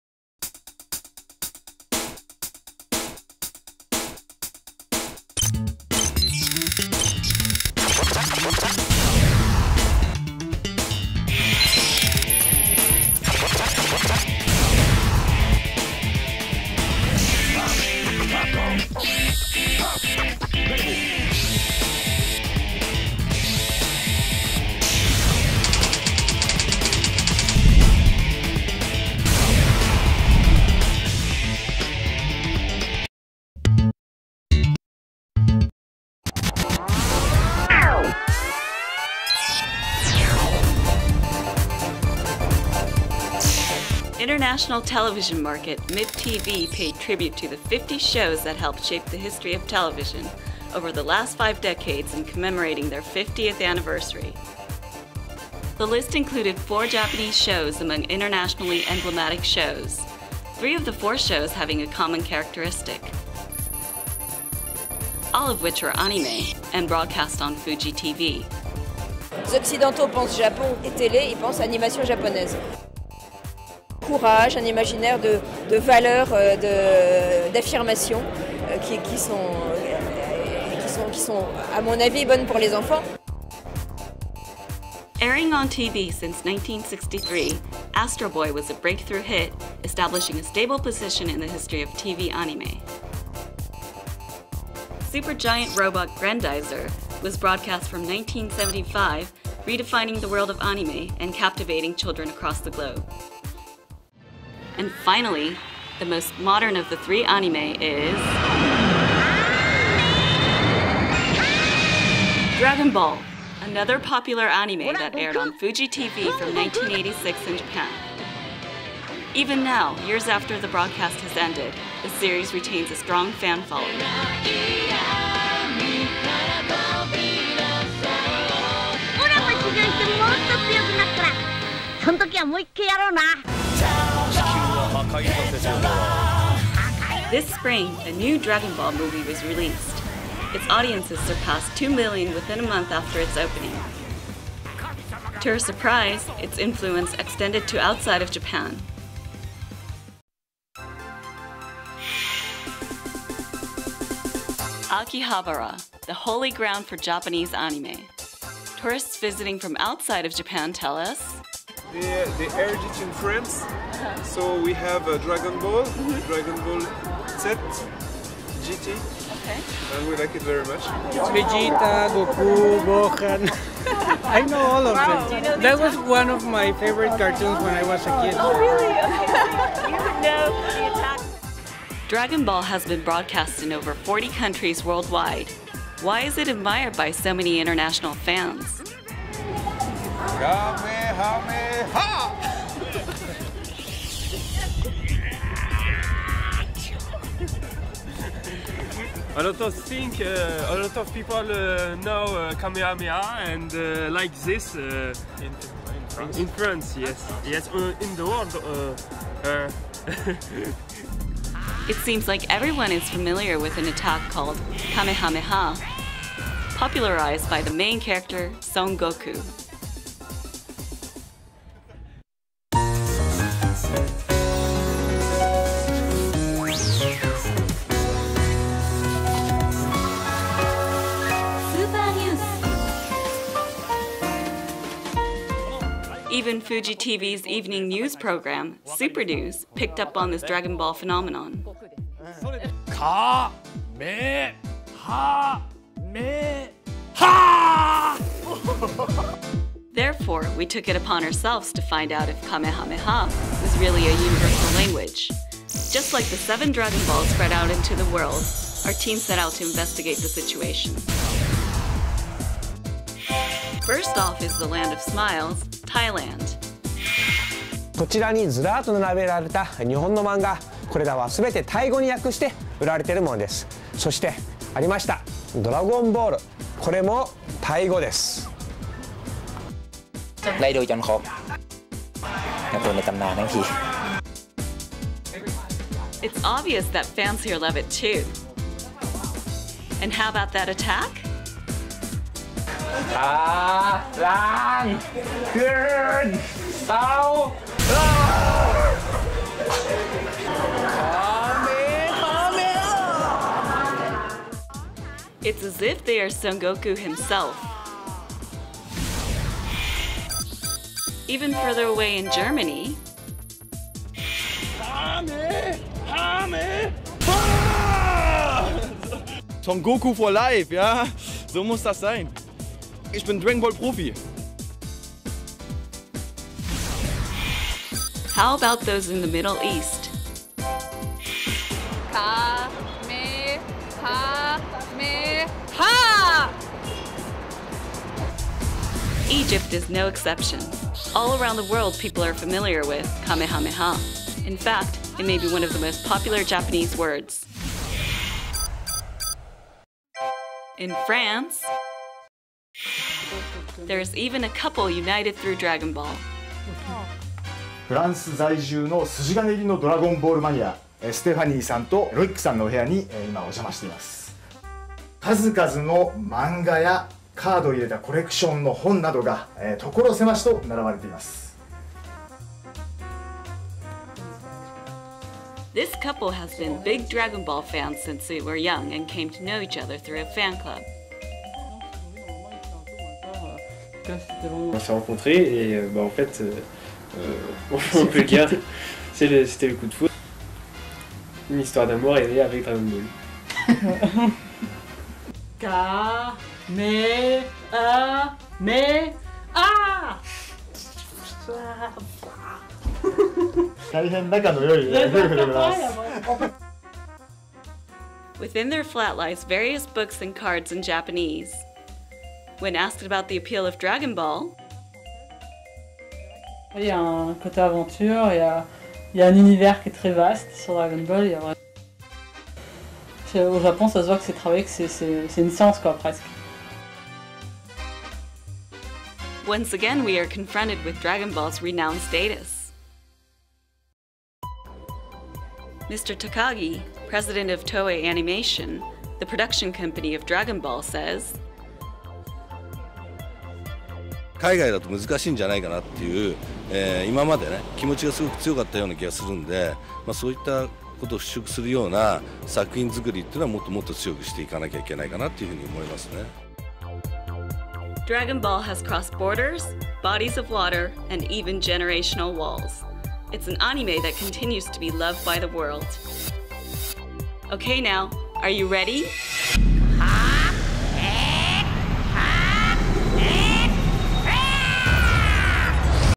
Dragon Ball worldwide "Kamehameha" Phenomenon 【Fuji TV Official】
For more Fuji TV videos in English, check out our official
International television market MIPTV recently announced the 50 shows that helped shape the history of television over the last five decades in commemorating their 50th anniversary. From Japan, 4 shows were selected including 3 Anime series that were broadcast on Fuji TV: Astro Boy, Grendizer and Dragon Ball. Of the 3, the most modern Anime series Dragon Ball perhaps has the widest range of fans, having been broadcast in over 40 countries. Why is it that it is admired by so many people around the world? We took it upon ourselves to find out why as well as the truth about Son Goku's attack method, "Kamehameha".